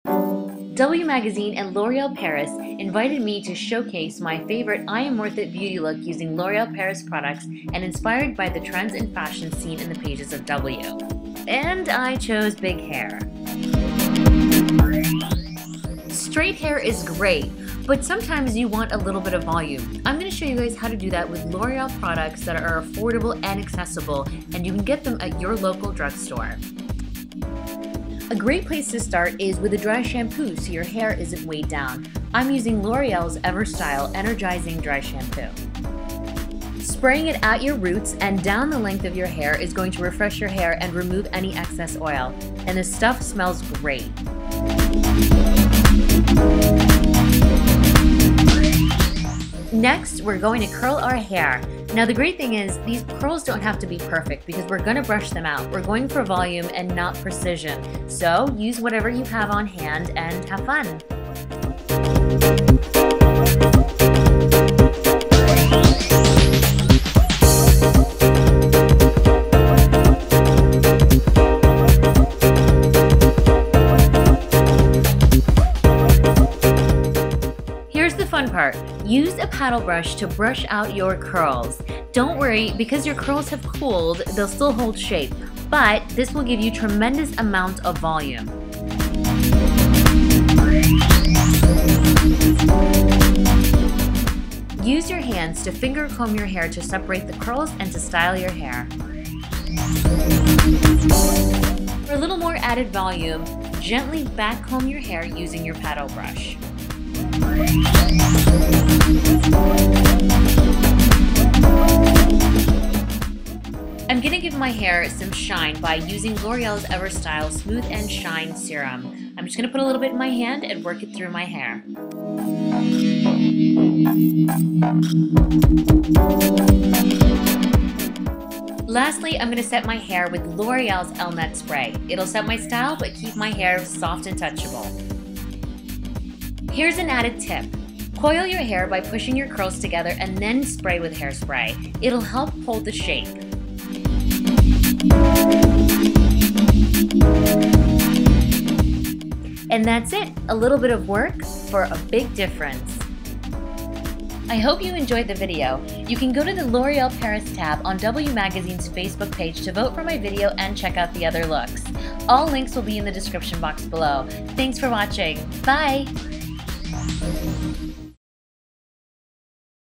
Voluminous Hair for W Magazine + L'Oreal Paris- Because You're Worth It
I had so much fun making this video and was honored to be to asked to participate, as they chose women who "embody the epitome of female empowerment and true confidence". Music to my ears :)

I'll be doing a step-by-step tutorial for the makeup. And, as requested, different types of curls with a curling wand. I'll teach you the Nur way...all technical and stuff ;) Thank you for everything, none of this is possible without you. Love you xo

Products Used:
L'Oreal Evertsyle Energizing Dry Shampoo
L'Oreal Evertsyle Smooth & Shine Serum
L'Oreal Elnett Satin Hairspray

Eye makeup: Tutorial coming soon, but I used L'Oreal Infallible eye shadows in Amber Rush and Eternal Sunshine for the extra sparke
Nails: L'Oreal Colour Riche in Royalty Reinvented